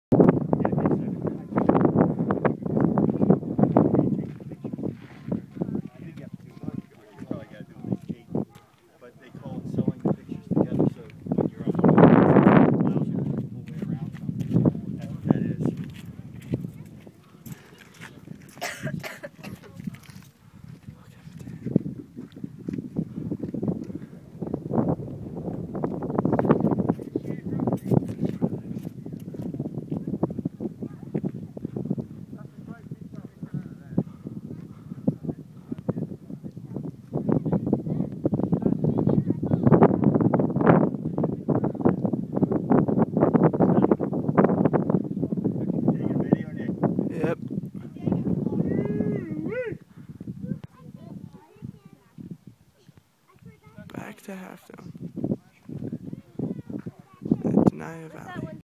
360 View from Sentinel Dome
On top of Sentinal Dome in the beautiful Yosemite National Park.